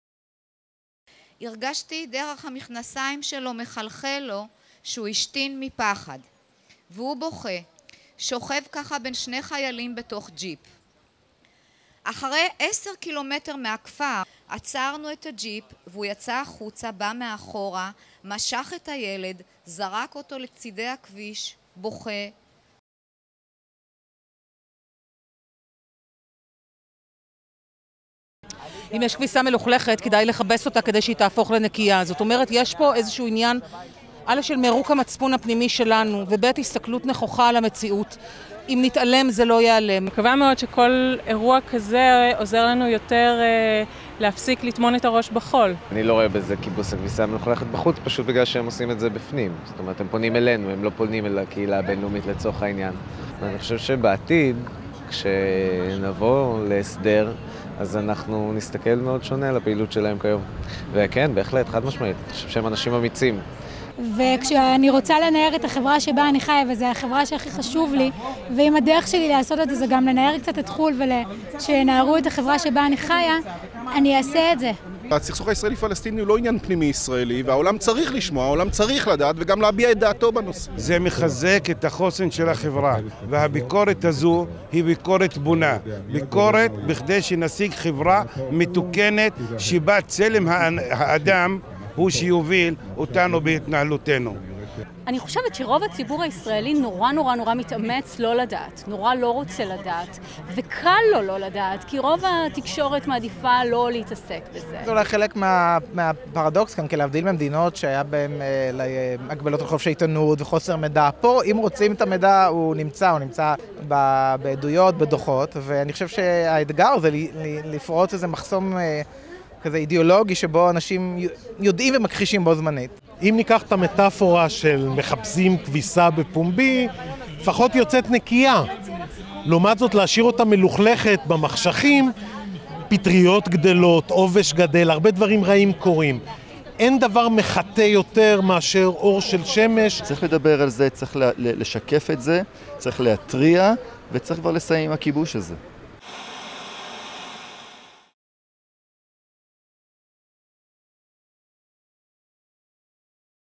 Should Dirty Laundry be Washed in Public?
On friday, june 6th, the organization "Breaking the silence", an organization that collects and publishes testimonies from soldiers who have served in the West Bank, Gaza and East Jerusalem, had an event to commemorate a decade since the establishment of "Breaking the Silence" and 47 years since the beginning of the Israeli occupation of the territories. In this event, testimonies were read in the center of Tel Aviv for 10 consecutive hours, and we asked people at the event "What is your attitude towards the claim that reading the testimonies is washing the dirty laundry in public?"